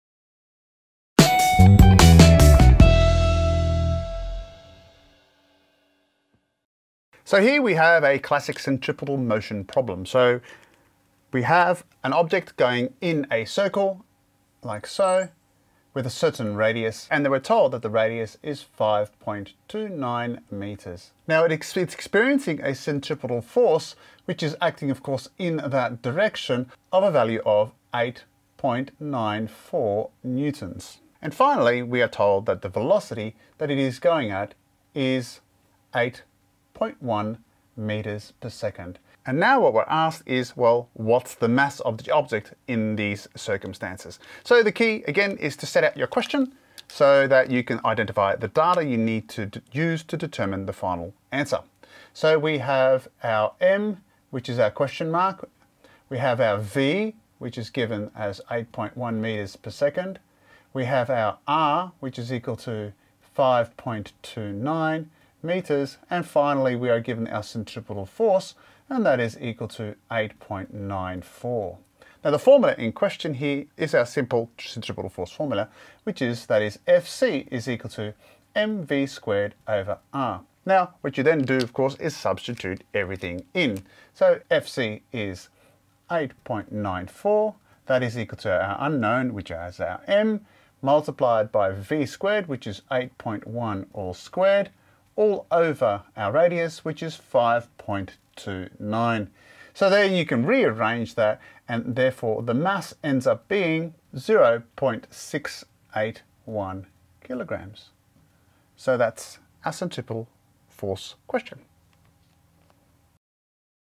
A sample circular problem problem with working
A sample problem with working on circular motion. Part of a series of worked solutions for my